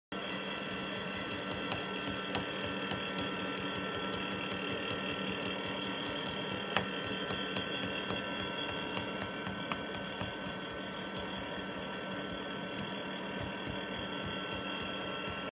1940's Electrovoice 805 Crystal Contact Microphone
Brief Oscilloscope demo of the EV805 mic.  These were originally intended to be installed as a hot-spot in acoustic guitars but quickly found uses in scientific applications as well.